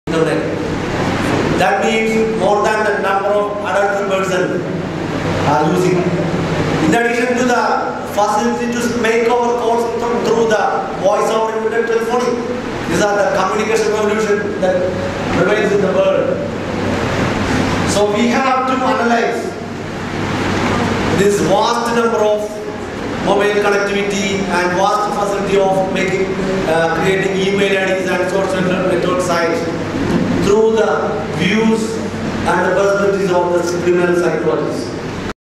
July 18, 2013 Awareness program on laws against ragging & cyber crimes
Mr. Francis Perera, Sub Inspector of Police, Cyber Cell, Ernakulam in the midst of Awareness program on Laws against Ragging & Cyber Crimes at Albertian Institute of Science and Technology - AISAT Technical Campus, Kalamassery